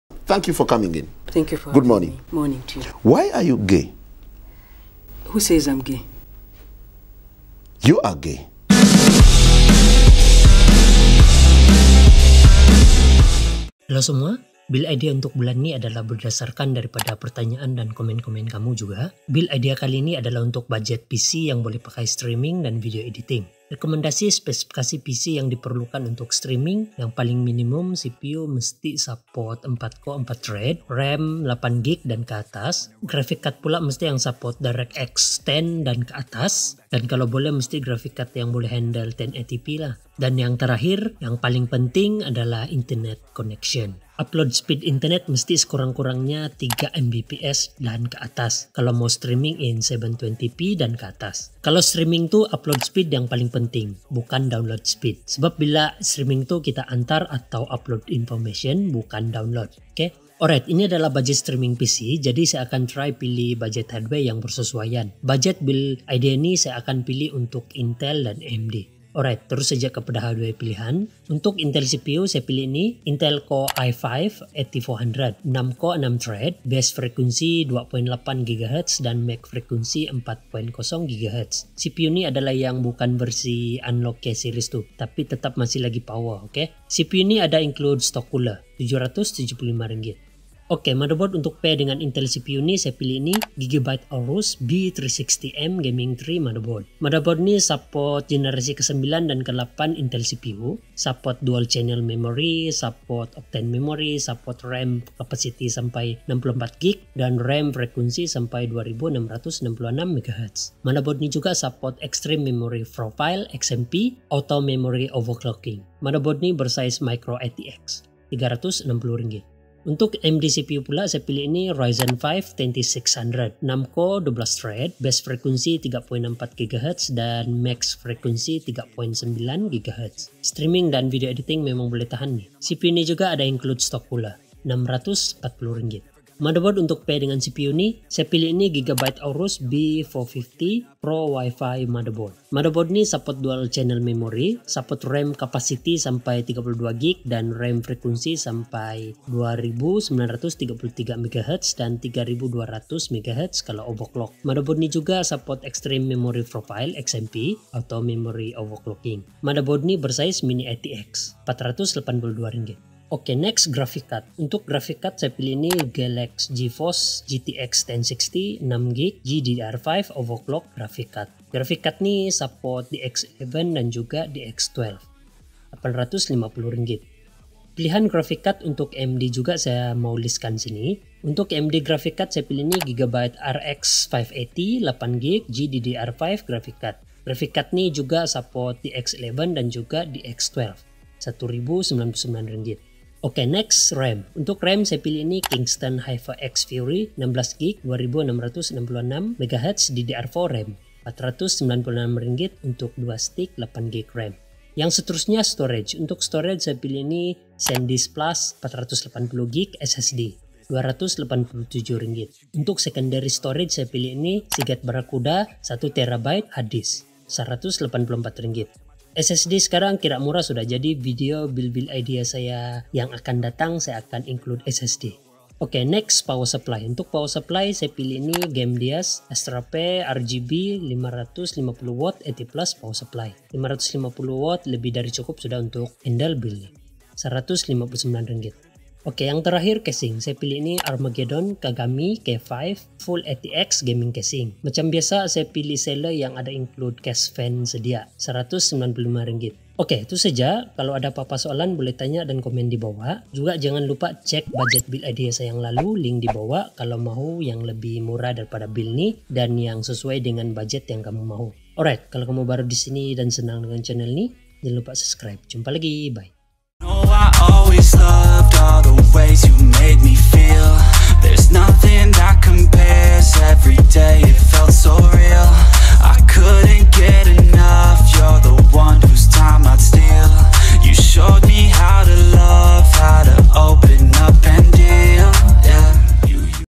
RM3,555 Bajet Streaming PC「February 2019」
Streaming, Gaming dan workstation PC Build idea. link utk setiap hardware yg dipilih ada dibawah.

Gamdias ASTRAPE E1-550W RGB 550W 80 Plus Power Supply

ESTIMATE BAJET KESELURUHAN
Intel build
dgn GTX 1060 = RM3,307
dgn RX580 = RM3,556

AMD build
dgn GTX 1060 = RM3,294
dgn RX580 = RM3,543

tambahan kalau mahu custom cooler
Cooler Master MasterAir MA410P CPU Cooler RM119.00

*harga mungkin atau akan berubah / berbeza selepas masa dan tarikh video ini dibuat. * ini juga termasuk stok item dari penjual itu sendiri. (jika link diatas memberitahu anda bahawa "items is no longer available", sila gunakan bar carian untuk item yang dikehendaki)

PENDEDAHAN / DISCLOSURES: All Genius links are tied to my Lazada / involveasia / productlink Associate account, from which I earn a small sales commission.